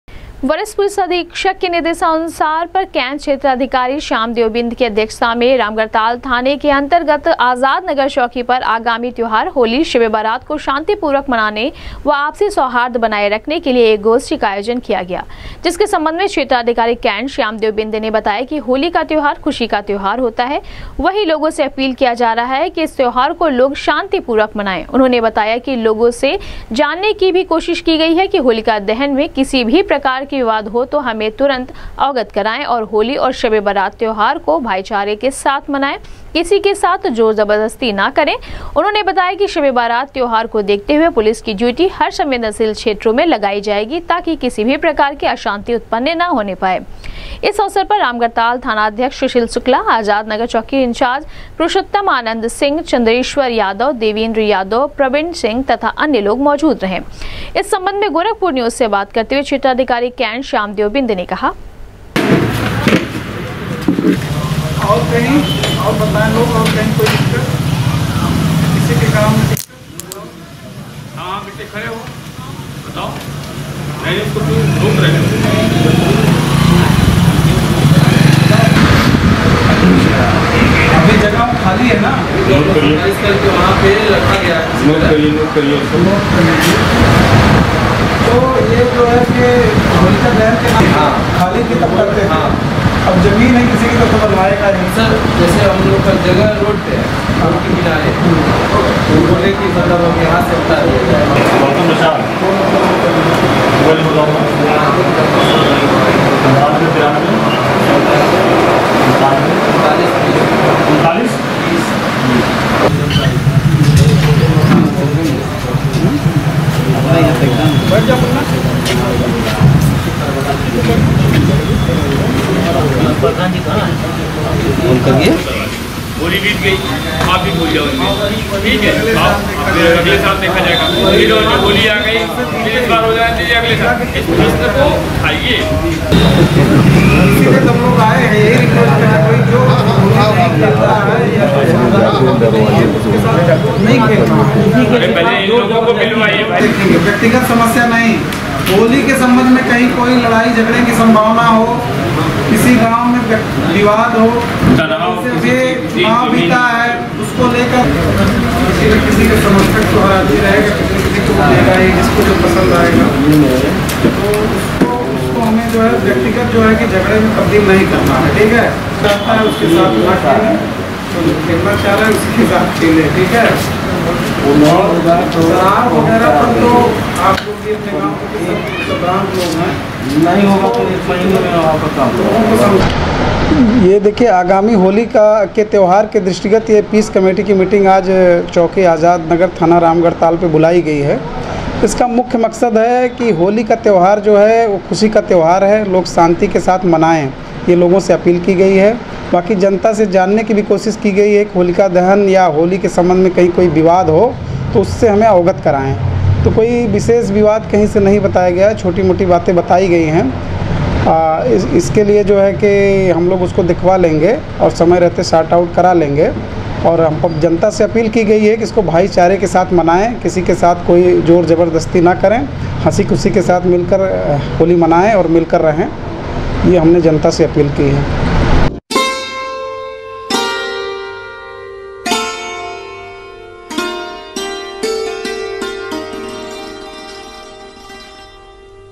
आगामी त्यौहार होली,शबे बारात को शांतिपूर्वक व आपसी सौहार्द पूर्वक मनाने के लिए सीओ कैंट
आगामी त्यौहार होली,शबे बारात को शांतिपूर्वक व आपसी सौहार्द पूर्वक मनाने के लिए सीओ कैंट श्याम देव बिंद की अध्यक्षता में आजाद नगर चौकी पर गोष्ठी का किया गया आयोजन, किया लोगों से शांति पूर्वक त्यौहार मनाने की अपील ।।